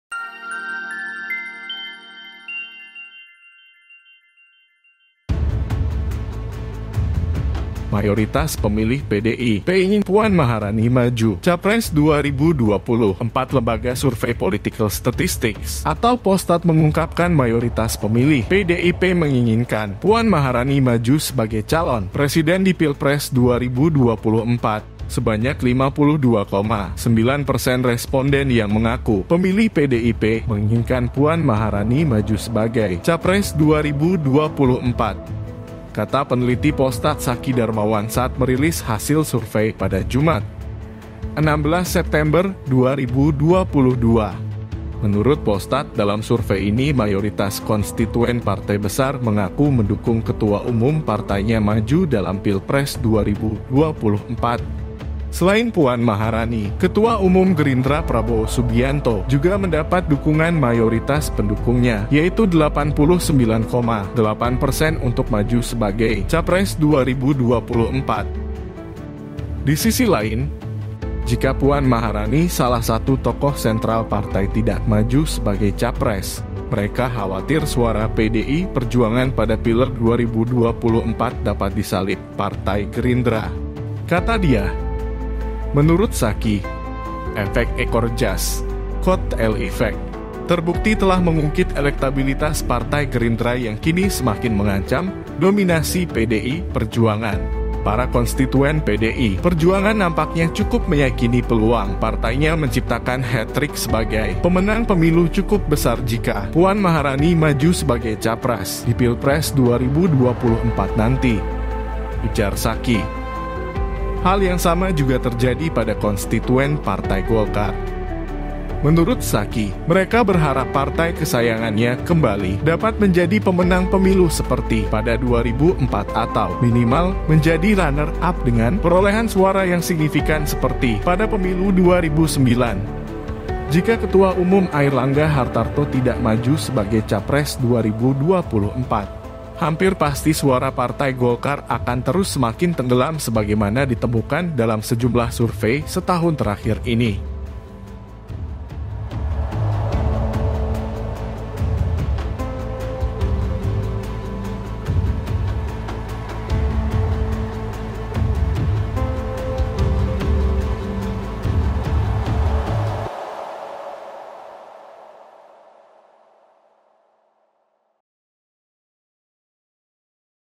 Pemilih PDIP Ingin Puan Maharani Maju Capres 2024
Lembaga survei Political Statistics atau Polstat mengungkapkan mayoritas pemilih PDIP menginginkan Puan Maharani maju sebagai calon presiden di Pilpres 2024.

"Sebanyak 52,9 persen responden yang mengaku pemilih PDIP menginginkan Puan Maharani maju sebagai capres 2024," kata peneliti Polstat Zaki Darmawan saat merilis hasil survei pada Jumat, 16 September 2022.

Menurut Polstat dalam survei ini mayoritas konstituen partai besar mengaku mendukung ketua umum partainya maju dalam Pilpres 2024.

Selain Puan Maharani, Ketua Umum Gerindra Prabowo Subianto juga mendapat dukungan mayoritas pendukungnya yaitu 89,8 persen untuk maju sebagai capres 2024.

"Sementara itu, sebanyak 68,7 persen responden yang menyatakan memilih Partai Golkar mengharapkan Airlangga Hartarto sebagai capres," kata Zaki Darmawan.